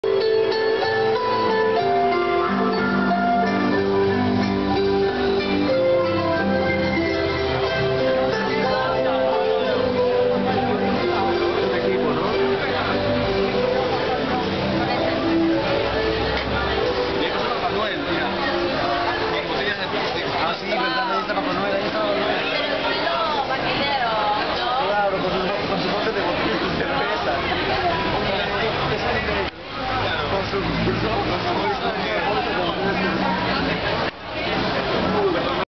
Bar Boteco da Garrafa, Río de Janeiro
Mirando la calle desde el bar Boteco da Garrafa, Avenida Mem de Sá, 77, Lapa, Rio de Janeiro. 15 de diciembre, 5.22pm.